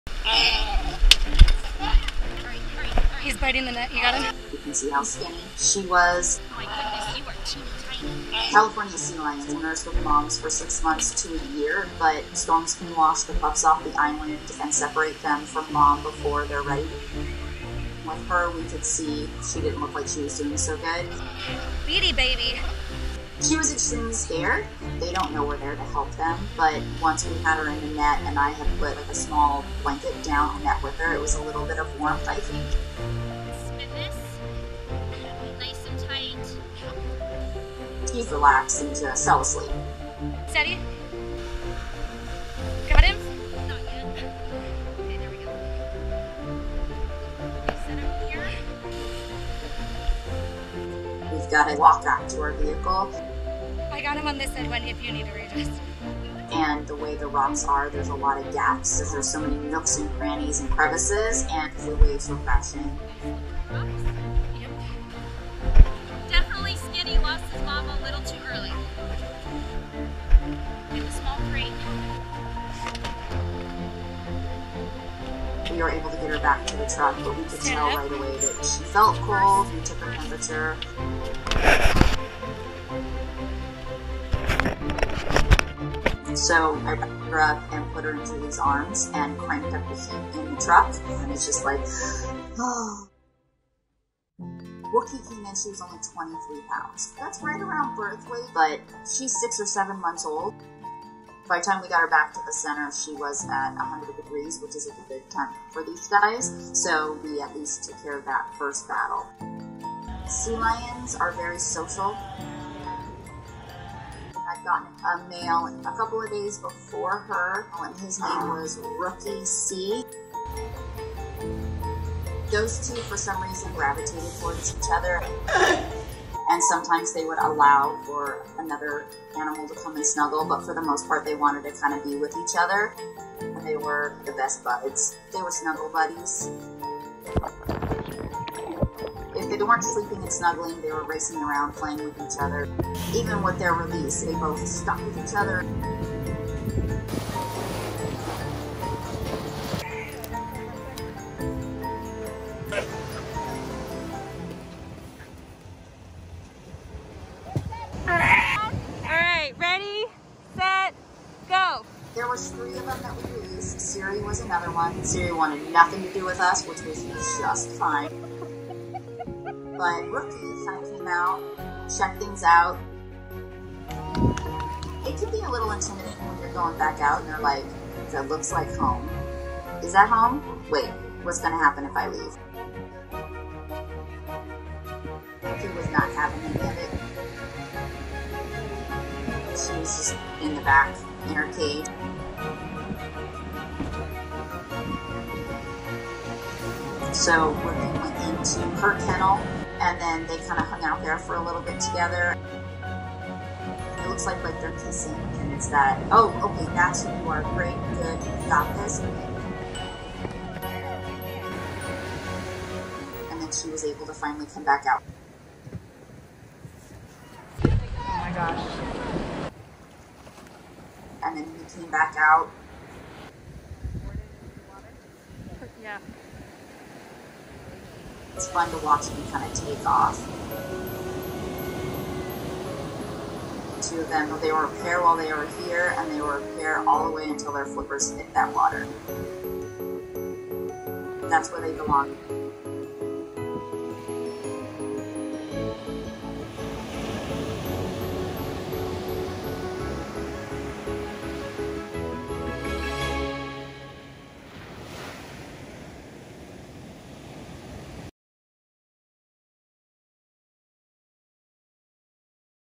Baby Sea Lion Scared To Go Back To The Ocean Gets A Little Help From Her Best Friend | The Dodo
Baby sea lion is scared to be released back into the ocean — until her best friend tells her to come with him.

To help these rescuers save more sea lions, you can support the Pacific Marine Mammal Center here: thedo.do/pmmc.